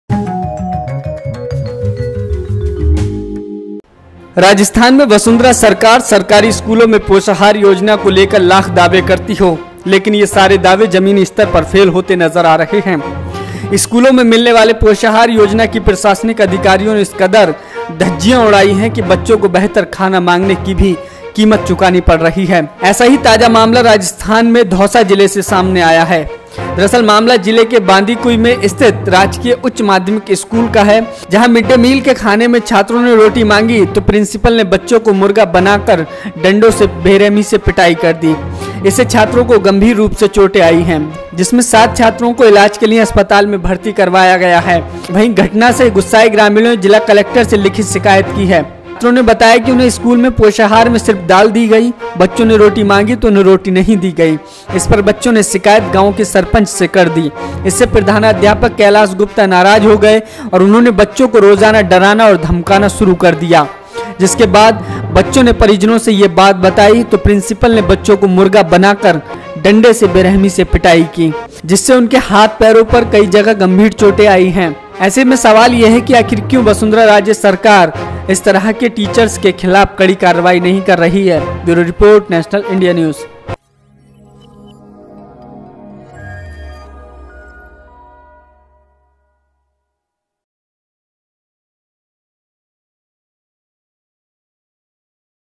वसुंधरा 'राज':सरकारी स्कूल के टीचर्स का बच्चों पर खौफनाक जुल्म/CM VASUNDHRA RAJE
Producer-Ambreen Shaikh
Editor- Susheel Kumar